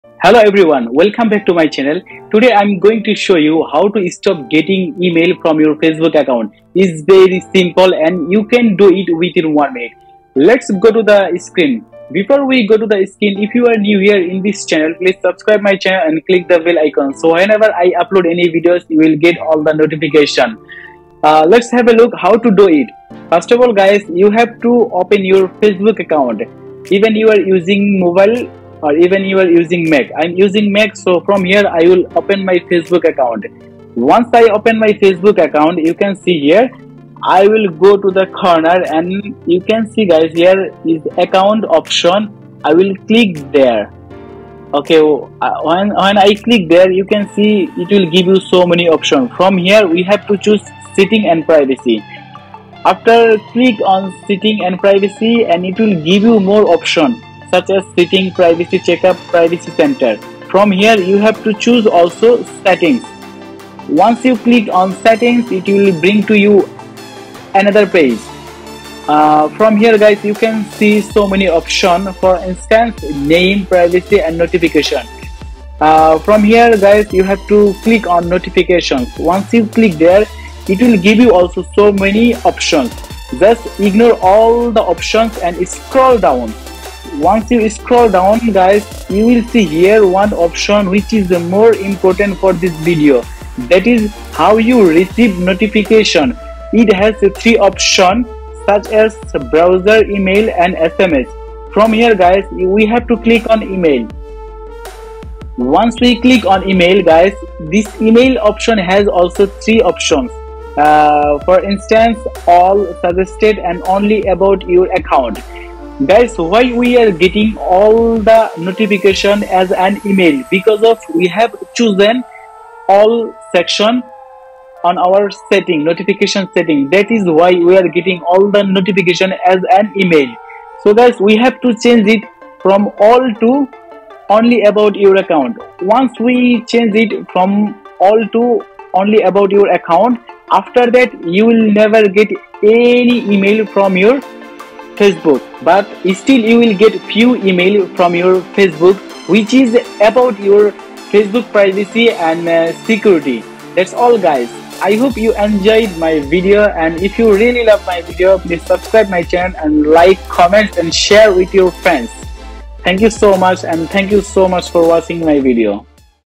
বিরক্তিকর ফেসবুক ইমেইল আসা বন্ধ করুন | How to stop Facebook notifications in Gmail/Email (2022)
HELLO EVERYONE,
In this video I have show you that how to stop getting email from Facebook .I hope you guys understood and enjoyed this video.

So whenever I upload any video ,you will get all the notifications.